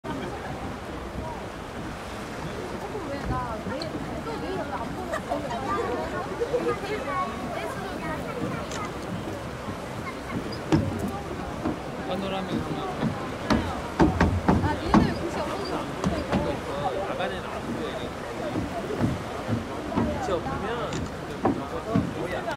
17 วิวอาทิตย์ตกทางฝั่ง Gwangalli Beach (BUSAN)
จุดชมวิว Dongbaekseom Lighthouse 동백섬 등대
มองเห็นทะเลแบบกว้าง ๆ และอาคาร Nurimaru APEC House
(누리마루 APEC하우스) ตอนเย็นวิวสวยงามมาก

ตอนเย็นอาทิตย์จะตกทางหาดควังอันลี Gwangalli Beach 광안리해수욕장

ถ่ายเมื่อ 3 มกราคม 2562